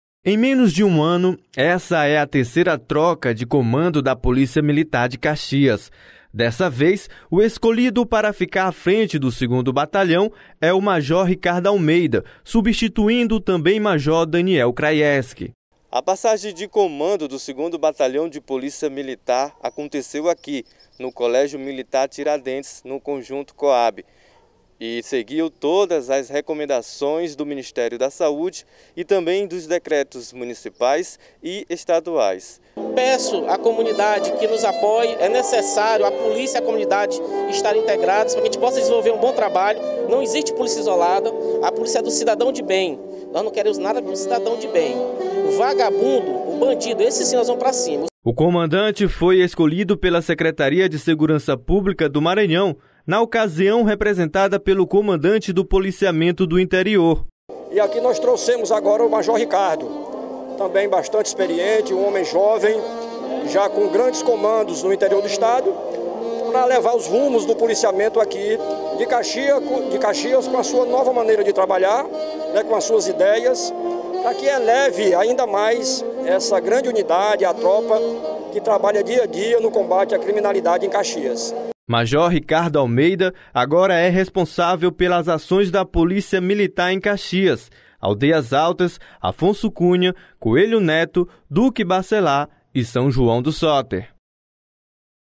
REALIZADA CERIMÔNIA DE TROCA DE COMANDO DA POLÍCIA MILITAR EM CAXIAS
Foi realizada a cerimônia que empossou o Major Ricardo Almeida como novo comandante do 2° Batalhão da Polícia Militar em Caxias.